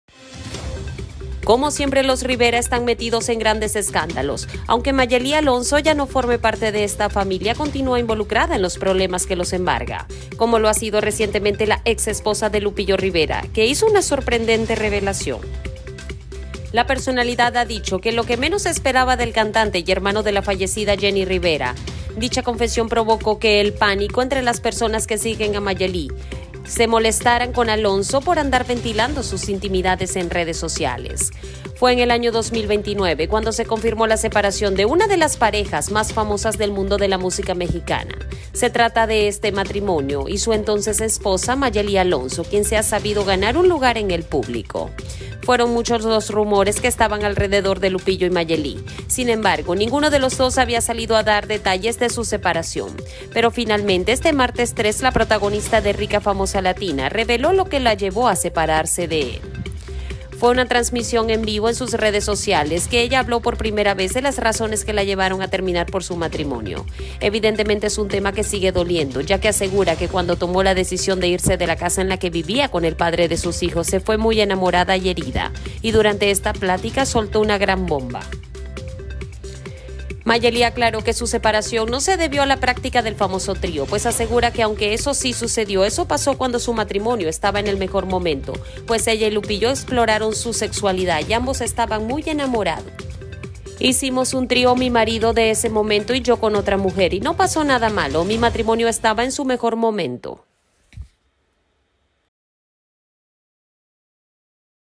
Mayeli Alonso confiesa algo IMPENSABLE de Lupillo Rivera
Como siempre los Rivera están metidos en grandes escándalos, aunque Mayeli Alonso ya no forme parte de esta familia continua involucrada en los problemas que los embarga. Como lo ha sido recientemente que la exesposa de Lupillo Rivera hizo una sorprendente revelación.

La personalidad ha dicho lo que menos se esperaba del cantante y hermano de la fallecida Jenni Rivera, dicha confesión provocó el pánico entre las personas que siguen a Mayeli.